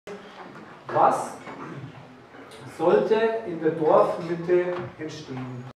Dorfentwicklung: Ortsmitte Langenpreising
Die Dorfentwicklung in Langenpreising ist seit Jahren nicht vorwärts gekommen. Was planen die Bürgermeisterkandidaten für die Ortsmitte in Langenpreising?